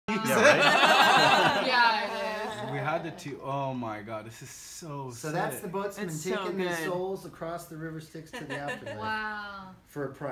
Mini-Charon | 3D Stroboscopic Zoetrope | By Peter Hudson
by Peter Hudson

Raised in Ben Lomond, CA and based in San Francisco, Peter Hudson, aka “Hudzo” self proclaims, “I am the director of the tallest and largest animated shorts.” Hudzo channels his technical and set-design expertise, curiosity, and creative passion into life-size 3D stroboscopic zoetropes. Hudson has spent 30 years in the world of stage and set design, working with the San Francisco Opera, Ballet, TV and film. In 2000, Hudson began creating his own larger scale sculptural installations, debuting his first major installation, Playa Swimmers, at Burning Man. In 2001, his work Possession garnered international attention when it was featured on the cover of the Italian edition of Rolling Stone magazine. Hudson’s work continued to evolve into 6 stroboscopic zoetropes: Sisyphish, Deeper, Homouroboros, Tantalus, Charon, and his most recent piece, Eternal Return.